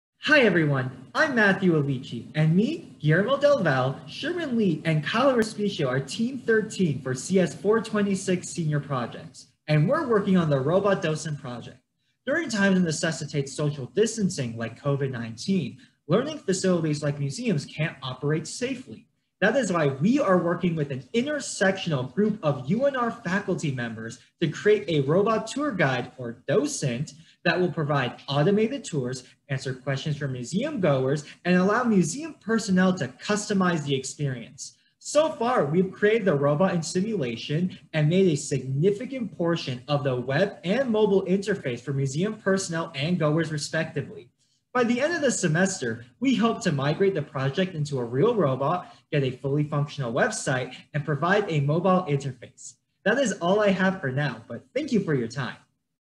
CSE 13 (Robot Docent), Innovation Day 2021
Team: Matthew Alighchi, Guillermo Del Valle, Kyle Respicio, Sherman Lee

External Advisors:

Casey Lynch, Department of Geography
Tyler Brownlow-Calkin, Department of Art
Vivian Zavataro, Lilley Museum

Our project, Robotic Docent, aims to create a robot museum tour guide that enables museums to still have tours in light of social distancing. Our project consists of a web application that allows museum staff to configure a robot, and a robot that obtains information from the web application and uses it to give a tour.